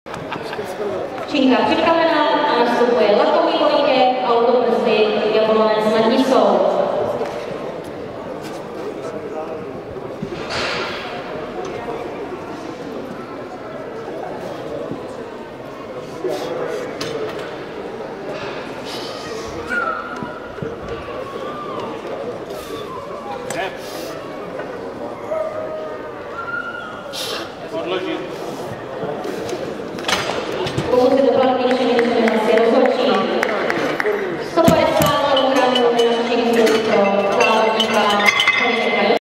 Lakomý Vojtěch (2001), DŘ3, 140 kg
3. Mistrovství Východních Čech v klasickém (RAW) silovém trojboji

Místo: Sportovní hala Na Střelnici, Svitavy

Federace: ČSST
Pořadatel: TJ Svitavy